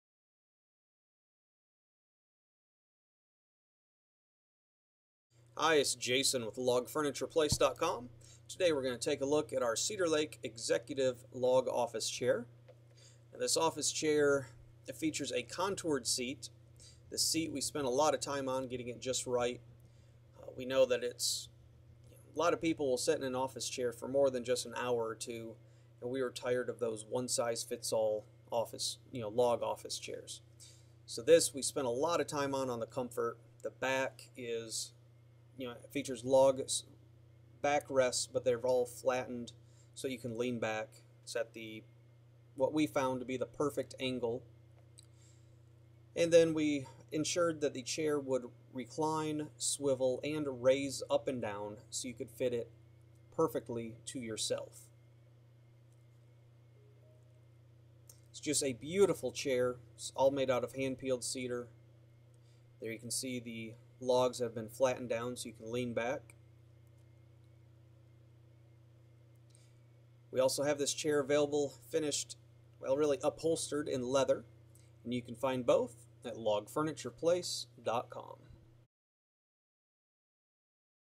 Log Office Chair a Log Executive chair from LogFurniturePlace.com
gives a closer look at their Cedar Lake log office chair.  This rustic executive chair features solid wood construction and comfortable seating.